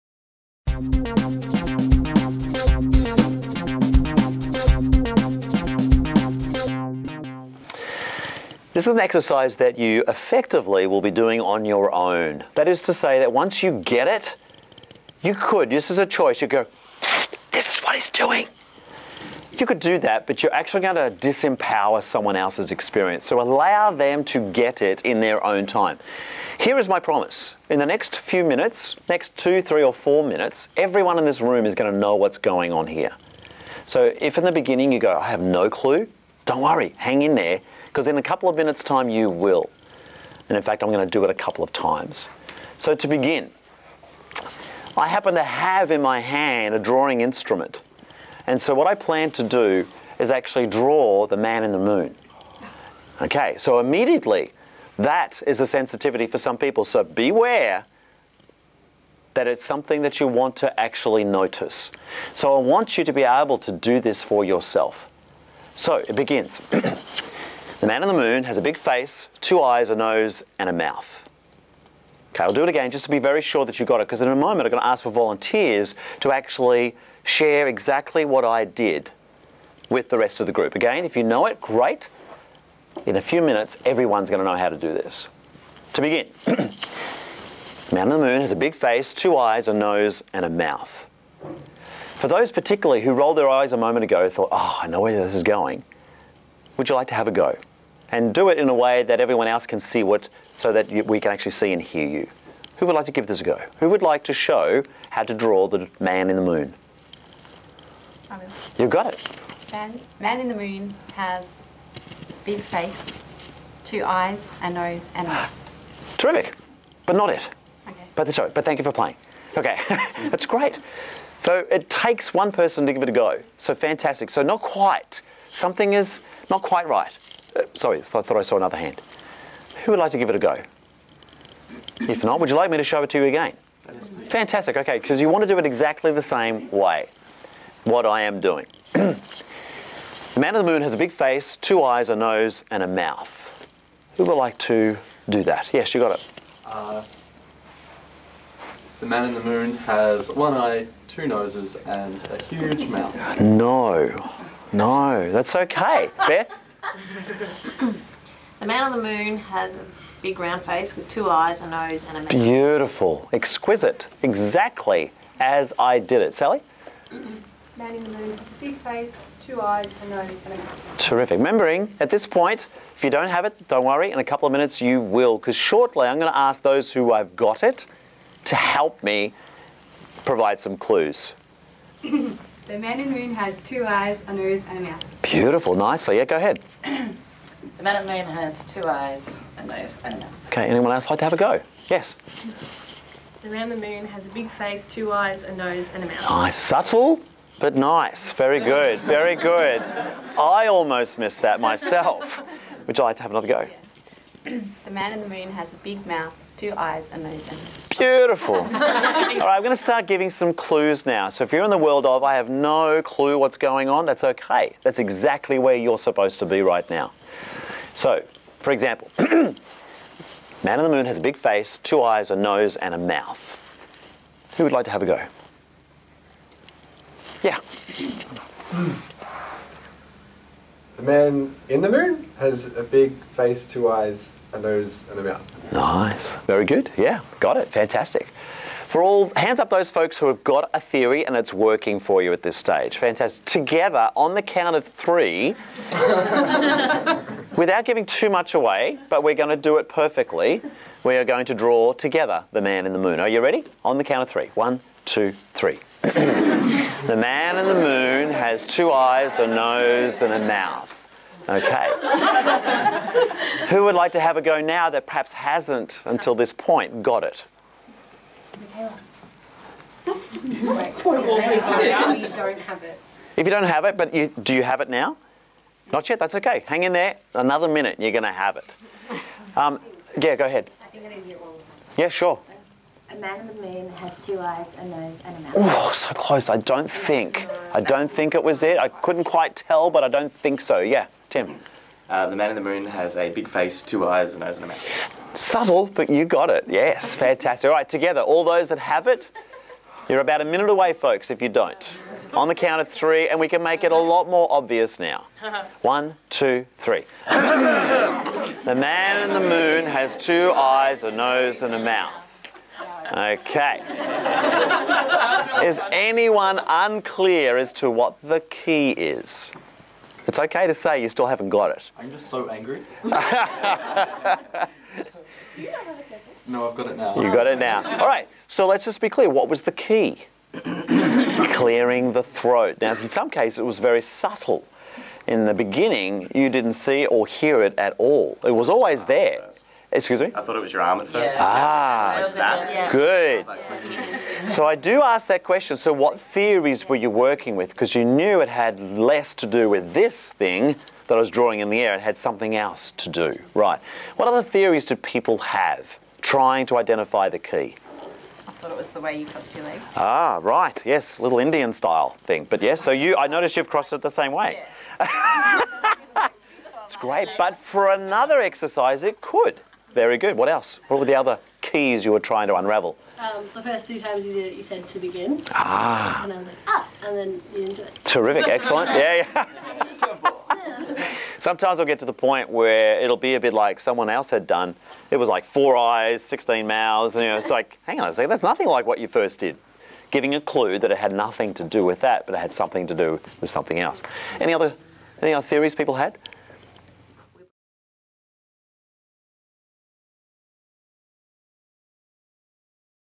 Frustratingly Simple Lateral Thinker - Man On The Moon
If you love presenting lateral thinking exercises, then watch this fun presentation of the classic Man in the Moon game to learn how to do it well. Focus on the introduction and then how Mark invites further input and engagement as the exercise gets closer to being solved.

UNLOCK 530+ FUN GROUP GAMES & ACTIVITIES